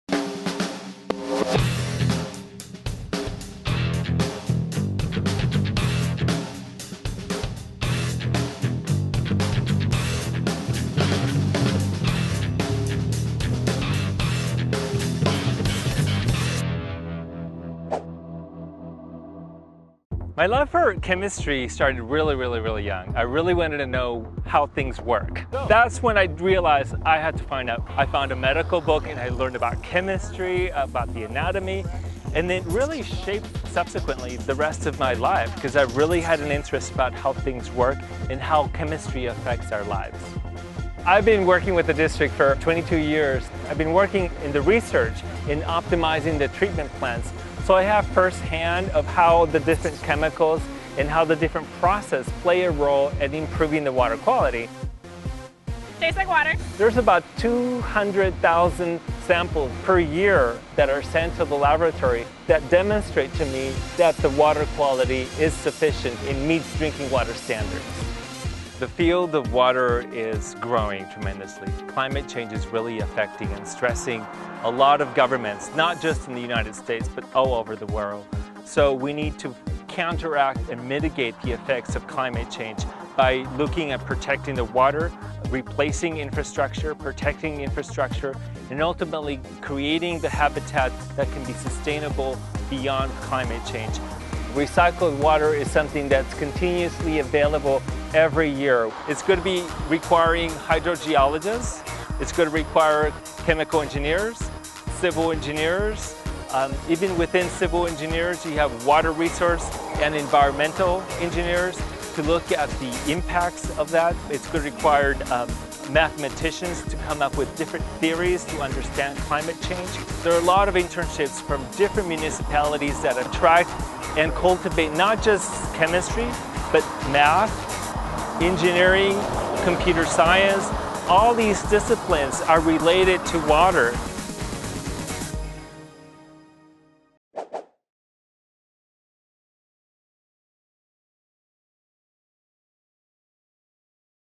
SciTrends - Water Treatment Careers
SciTrends explores the different careers in the water treatment industry.